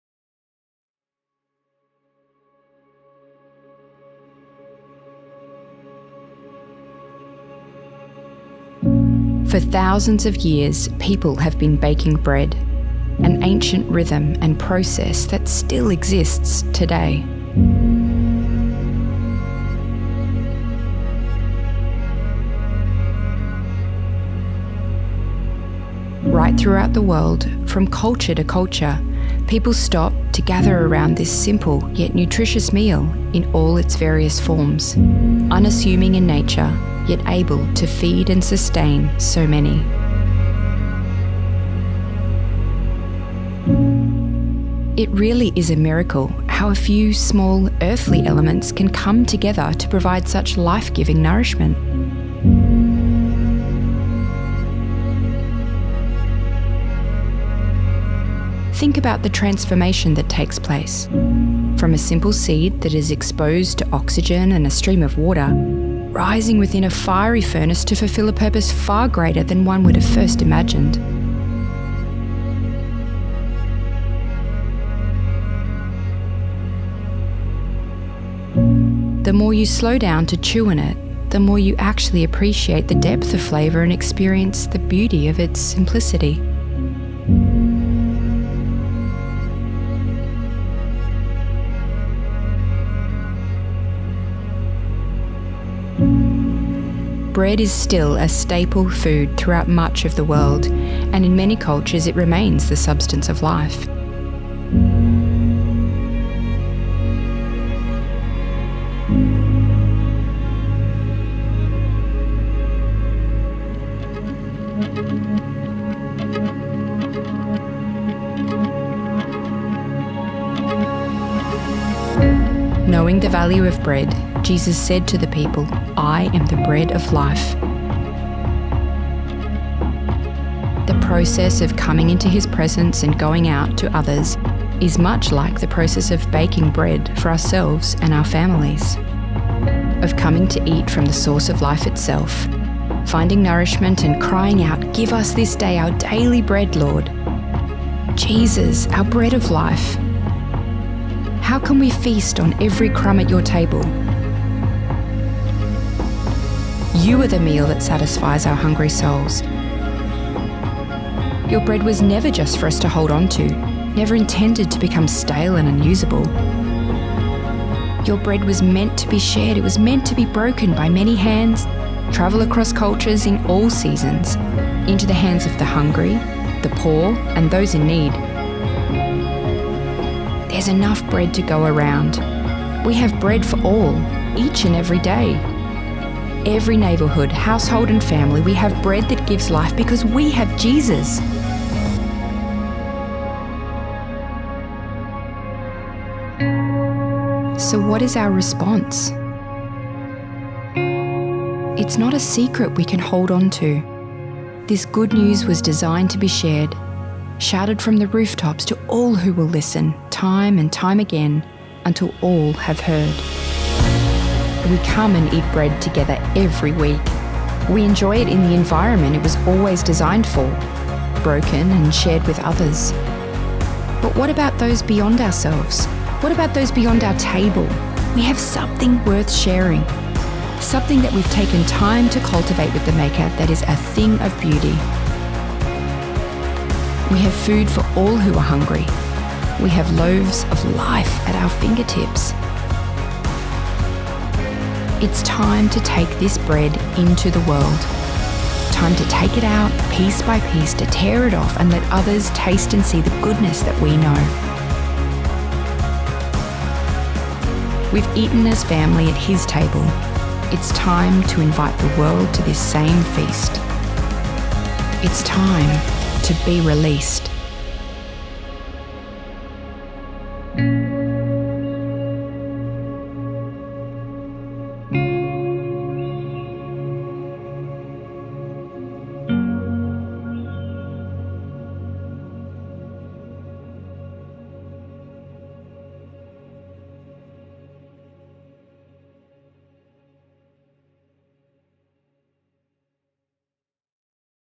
Bread // Vision 2020
As a community, we are so excited for the year that is ahead. We believe God is going to empower and release us to share a powerful message of love to everyone around us.